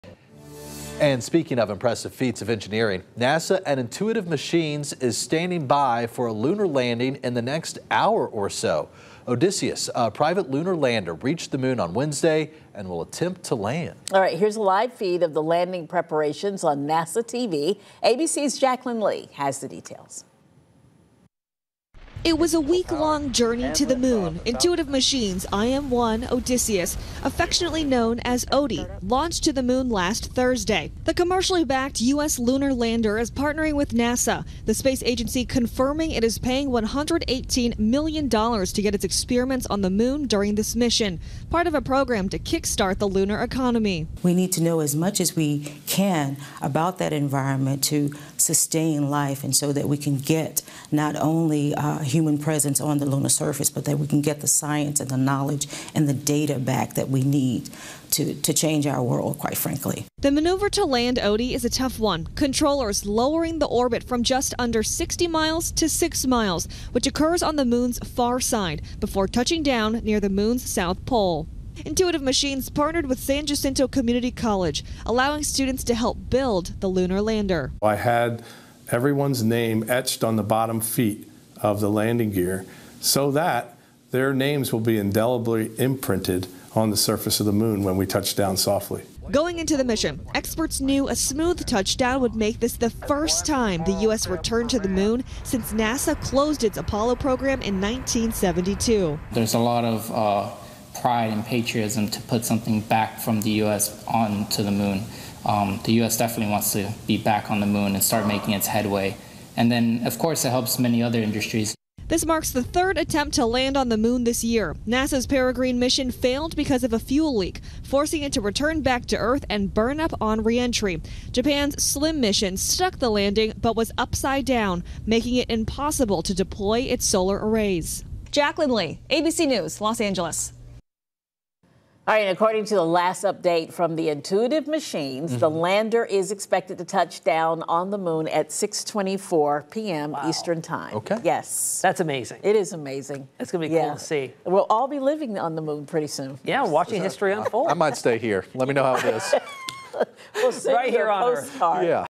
Odysseus, a private lunar lander, reached the moon on Wednesday and will attempt to land.
NASA and Intuitive Machines is standing by for a lunar landing in the next hour or so. Odysseus, a private lunar lander, reached the moon on Wednesday and will attempt to land. Here's a live feed of the landing preparations on NASA TV. ABC’s Jaclyn Lee has the details.